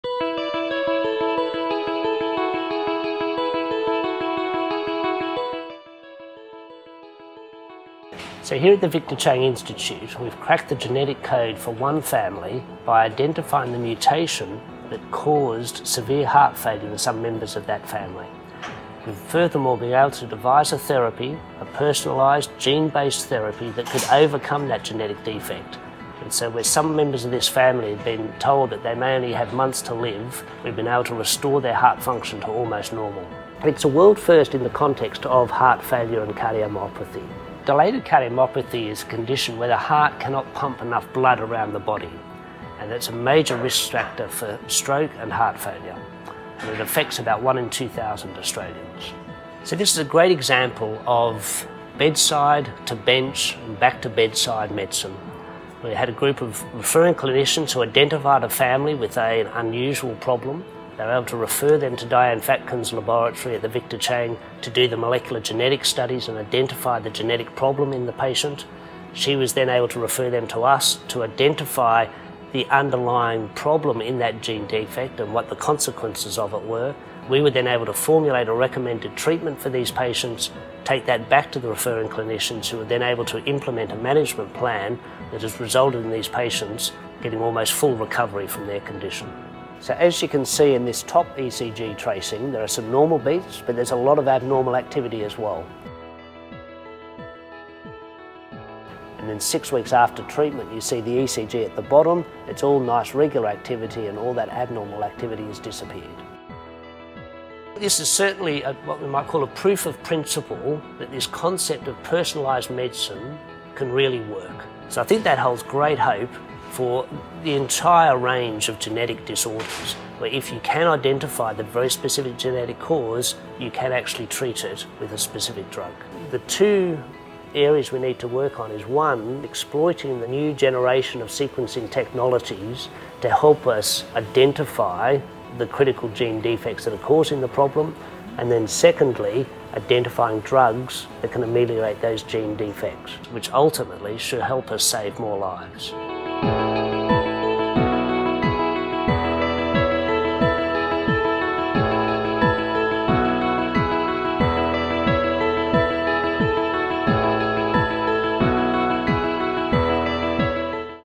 New era medicine: Reversal of a family's heart rhythm disorder
In a world first, Sydney researchers have used gene-based therapy to successfully reverse an inherited heart rhythm disorder in members of an entire family, some of whom had previously been told they had only months to live.

This gene-therapy, practised as part of the new era of personalised medicine, achieved remarkable results. The research is published in the Journal of the American College of Cardiology.

Rather than using conventional heart failure medicine, the researchers from the Victor Chang Cardiac Research Institute in Sydney, identified and treated the gene mutation that caused their heart rhythm disorder, dilated cardiomyopathy (DCM). Many patients with severe disease returned to full health within 6 months.

Find out more about this research and the work of the Victor Chang Institute